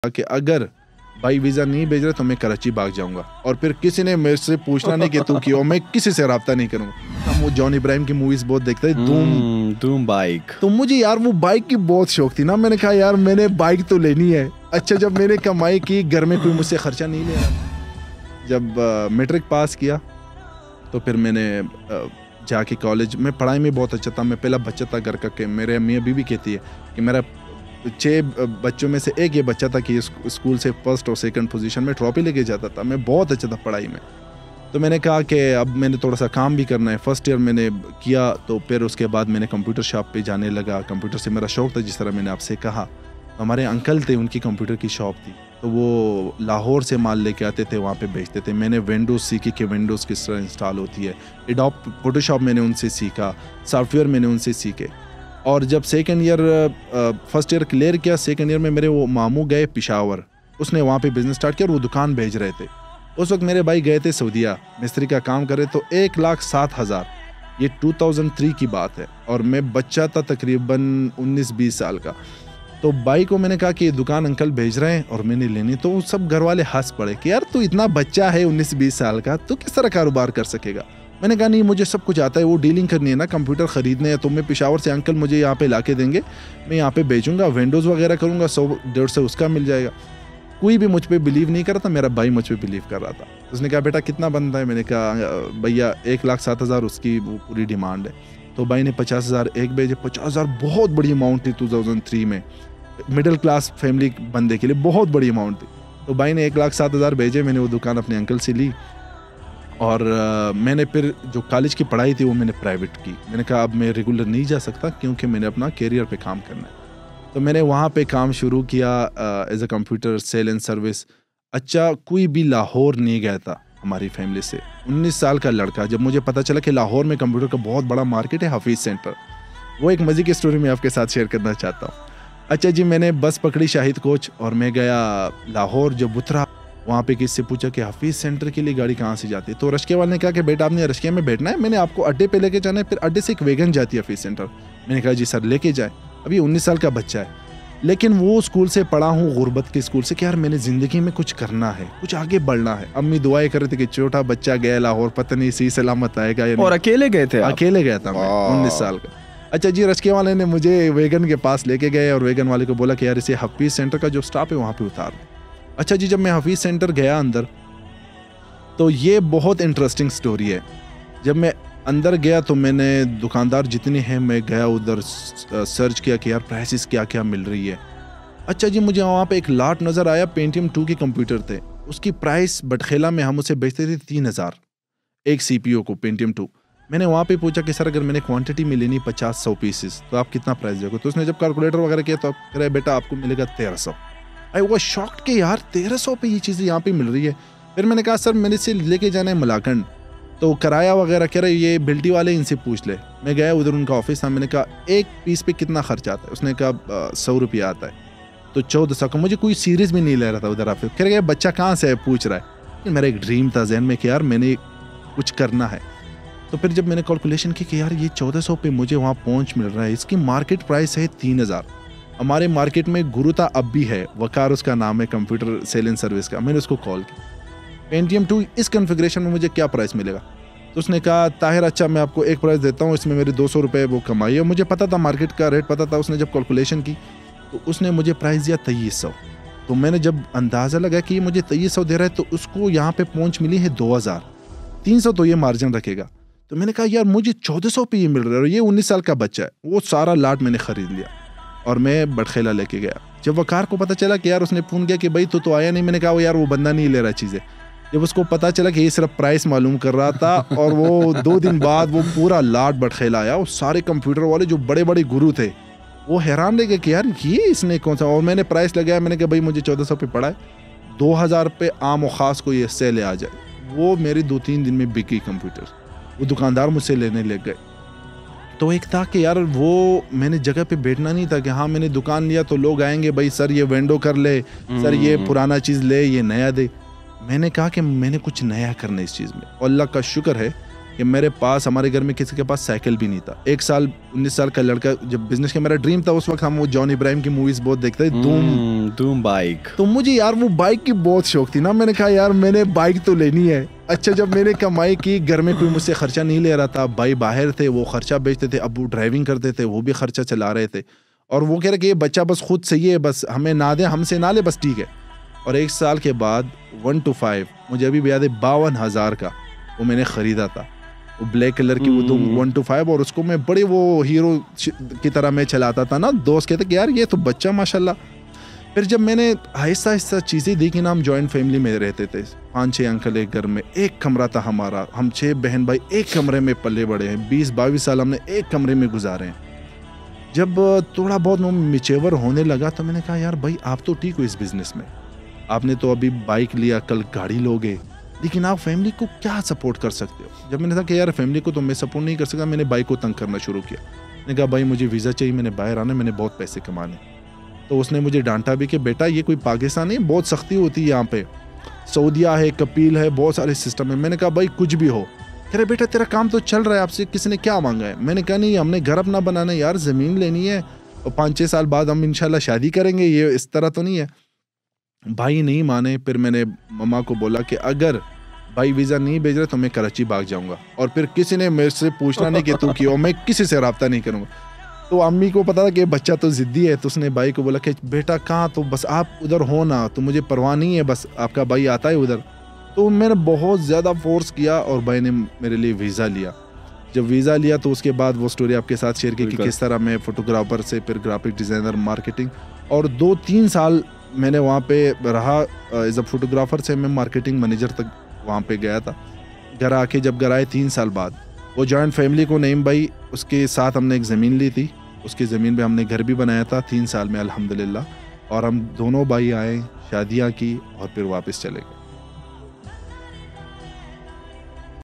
How I Started My First Business at Age Of 17 ? | Tahir Khan Videos
Why I Left my education and started my own business at age of 17.
Interesting real life story of Tahir khan.